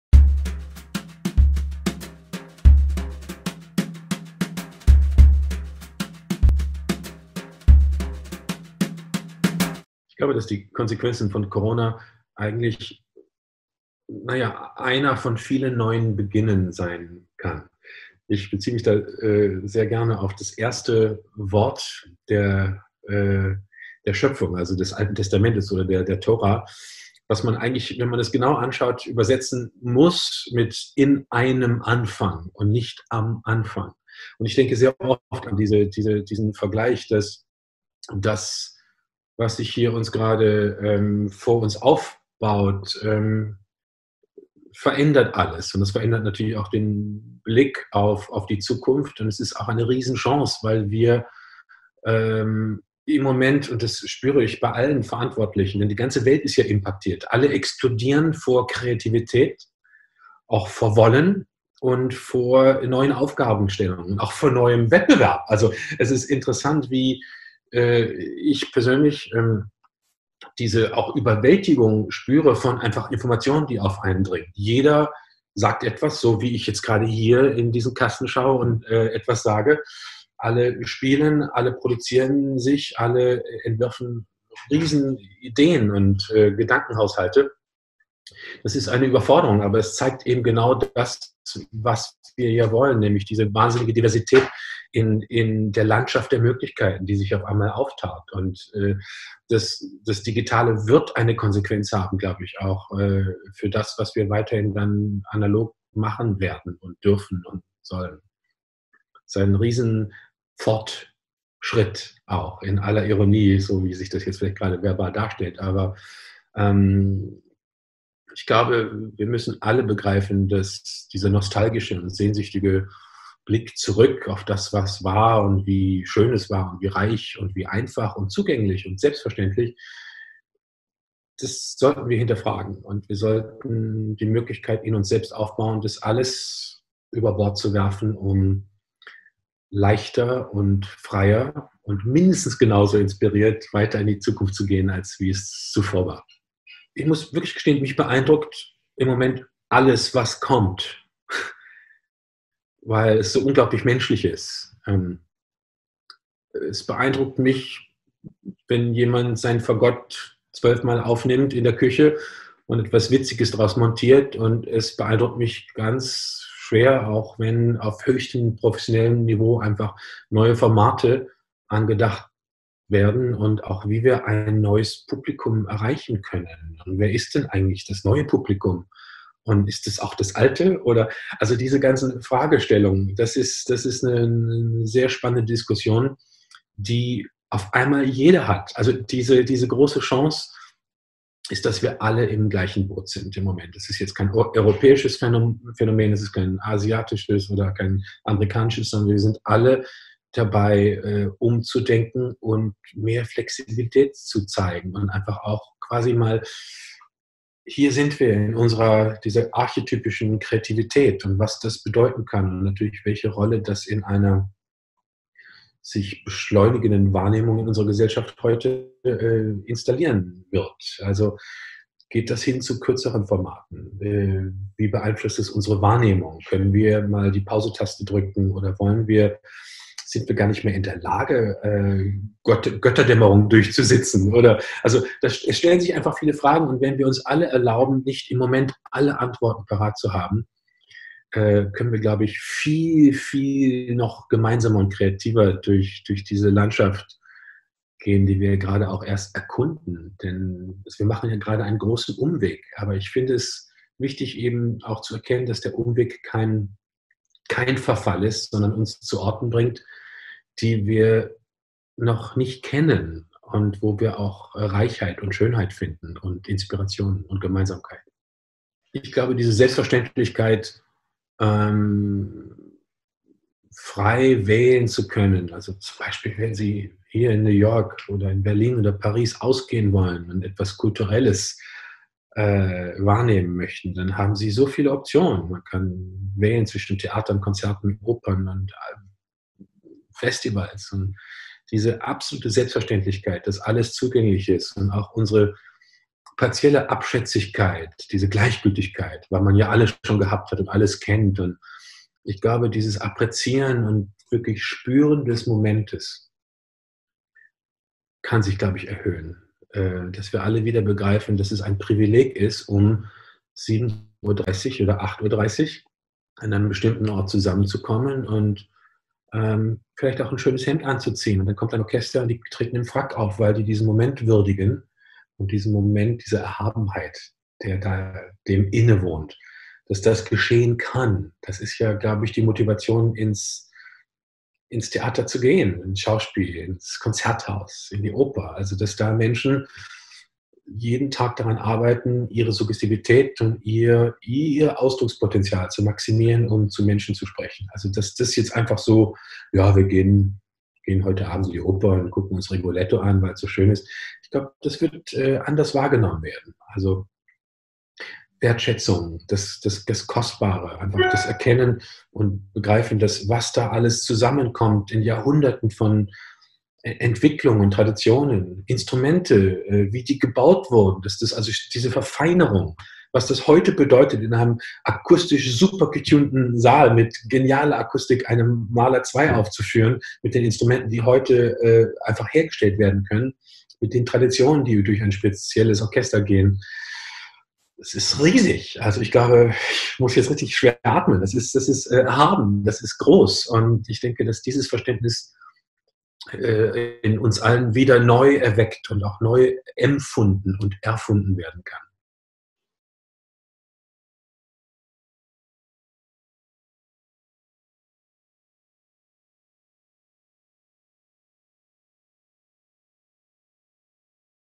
Corona-Talk mit Matthias Pintscher
Der 21. Juni ist traditionsgemäß der „Tag der Musik“, initiiert durch den Deutschen Musikrat. In den vergangenen Jahren haben Amateur- wie Profimusikerinnen und -musiker in ganz Deutschland rund um diesen Tag die Vielfalt der Musik gefeiert. In diesem Jahr ist fast alles anders. Wir fragen Musikerinnen und Musiker nach dem Stellenwert der Musik in Zeiten von Corona – und wie sich die Musikwelt verändern wird. Heute in Folge 3: Matthias Pintscher, Komponist und Dirigent